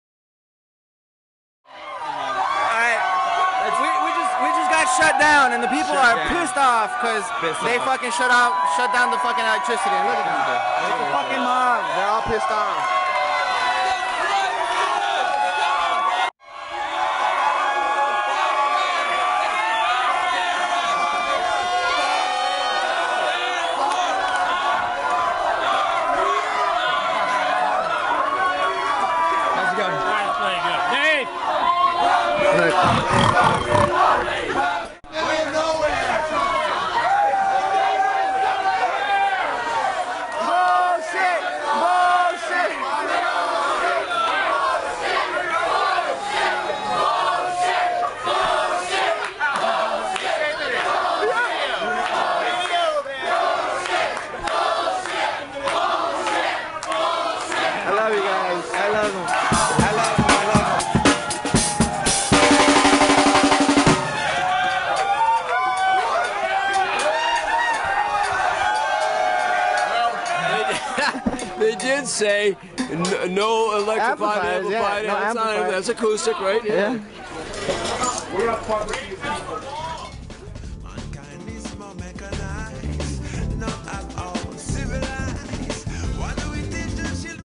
Riot at a Kēvens show in Vermont
There was some technical difficulties with the sound system at that event in Vermont causing me and crew to go on stage late. Security decided to shut it down while I am on stage, needles to say, my fans were not happy with that decision.
Video footage by Peter Ruprecht.